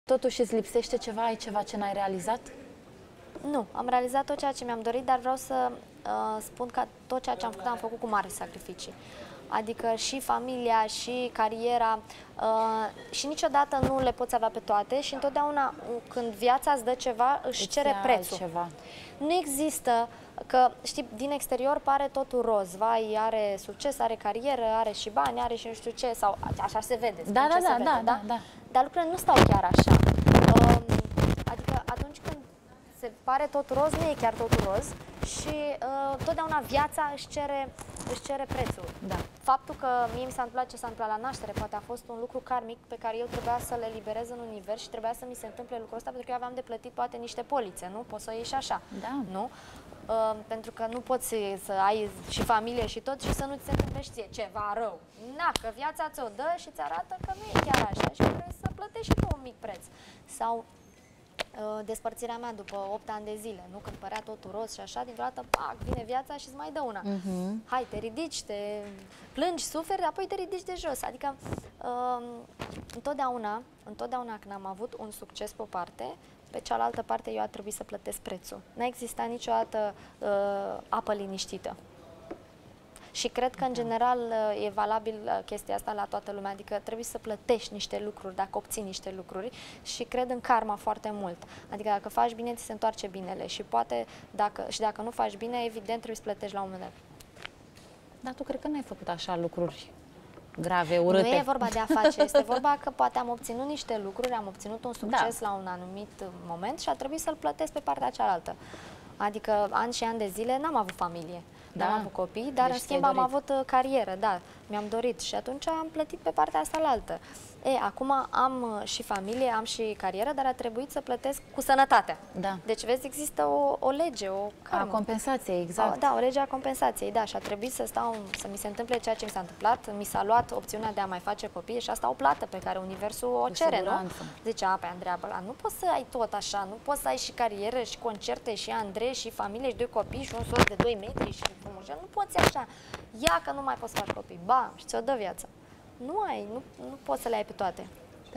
Andreea Bălan vorbește despre karma: ”Am plătit cariera și familia cu sănătatea”
Cântăreața care a fost la un pas de moarte în luna martie 2019 a mărturisit de ce nu e supărată pe faptul că nu mai poate face copii, deși e tânără. Decizia a fost luată de medicii care i-au salvat viața în momentul în care Andreea Bălan a făcut stop cardio-respirator, după cezariană.